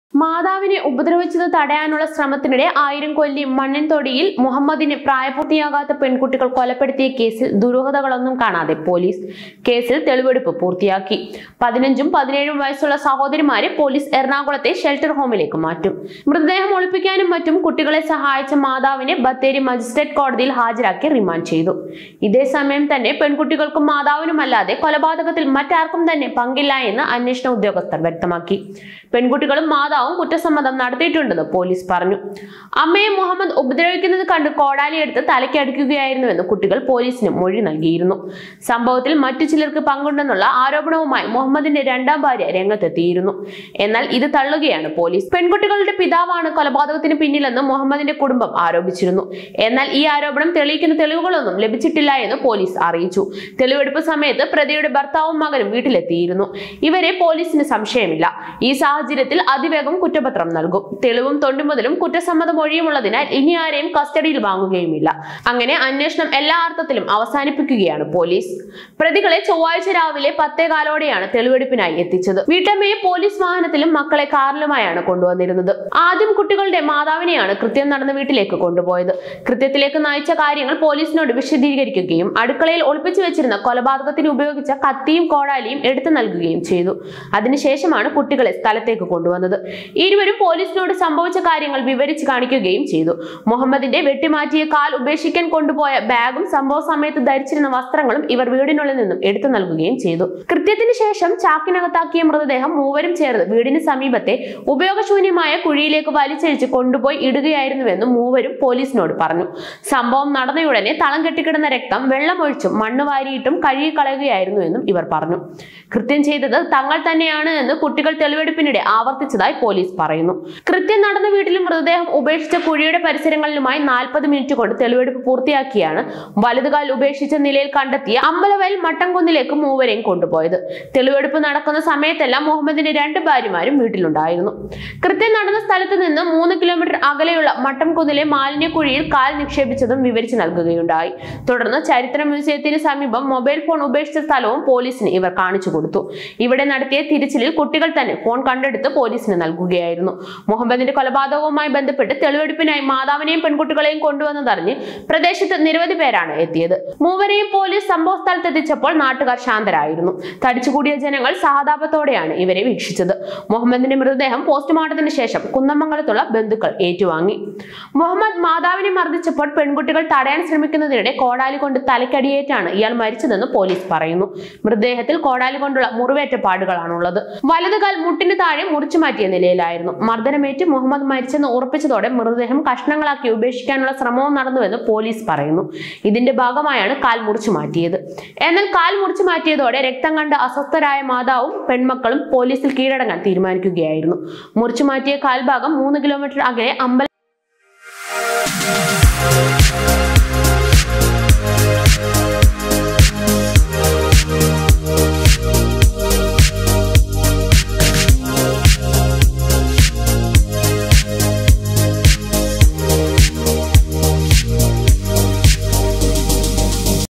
മുഹമ്മദിനെ കൊന്നത് പെൺകുട്ടികളും അമ്മയുംചേർന്ന്' ; പോലീസ്
മാതാവിനെ ഉപദ്രവിച്ചതു തടയാനുള്ള ശ്രമത്തിനിടെ ആയിരംകൊല്ലി മണ്ണിൻതൊടിയിൽ മുഹമ്മദി(68)നെ പ്രായപൂർത്തിയാകാത്ത പെൺകുട്ടികൾ കൊലപ്പെടുത്തിയ കേസിൽ ദുരൂഹത കാണാതെ പൊലീസ്. കേസിൽ തെളിവെടുപ്പ് പൂർത്തിയാക്കി. 15,16 വയസ്സുള്ള സഹോദരിമാരെ പൊലീസ് എറണാകുളത്തെ ഷെൽറ്റർ ഹോമിലേക്കു മാറ്റും.
AMBALAVAYAL